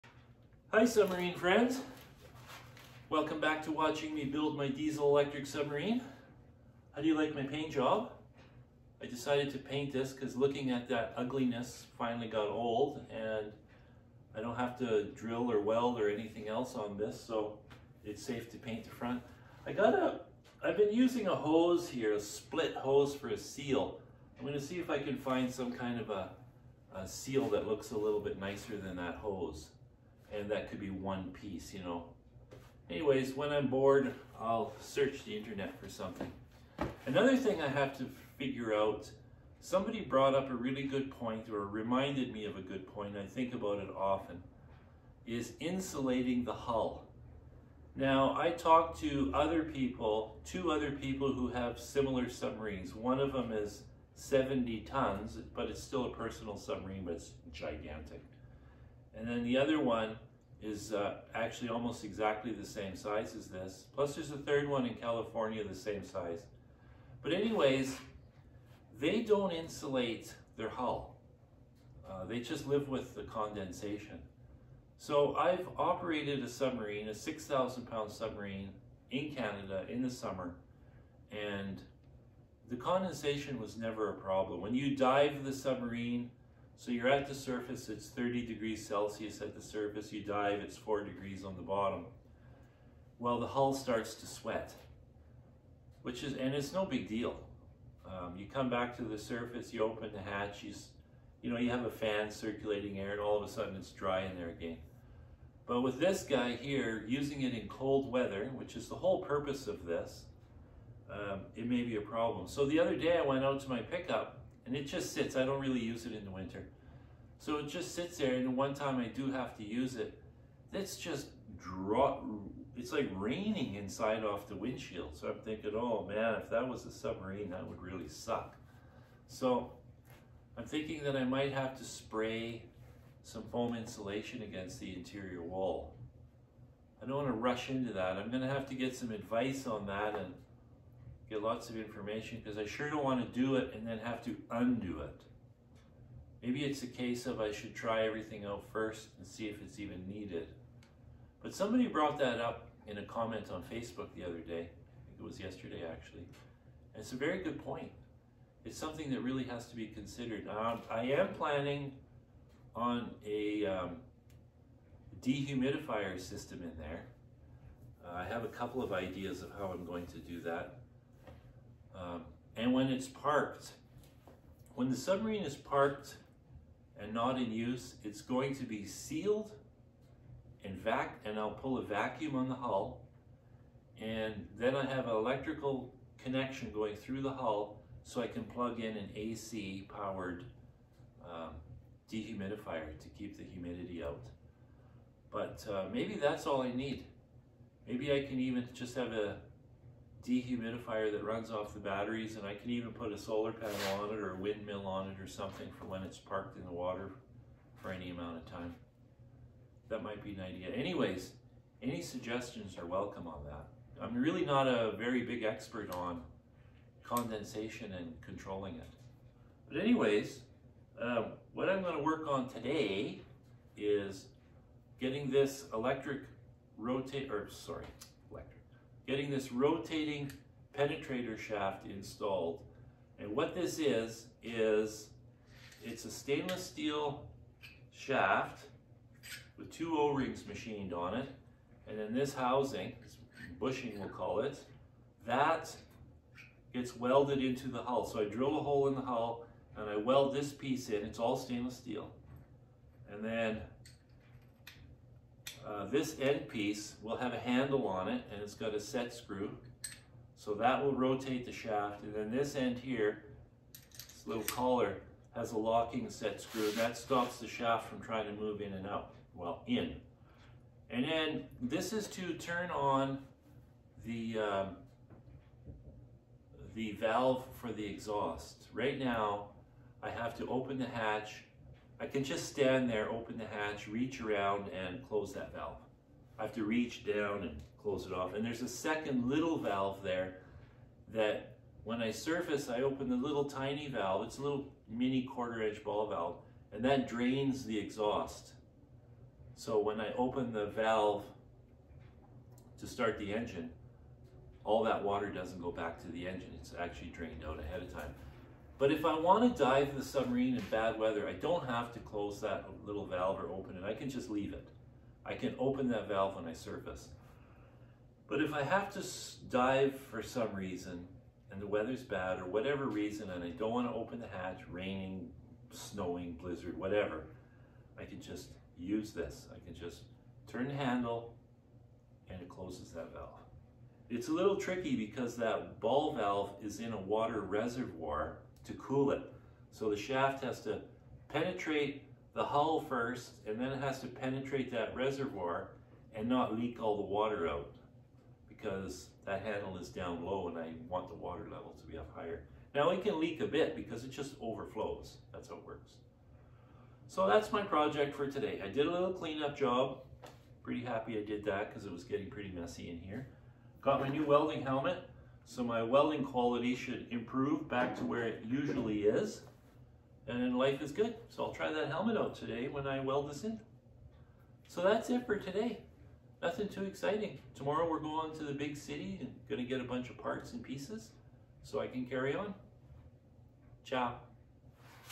building my diesel electric submarineJanuary 13, 2026
Exhaust valve rotating shaft install.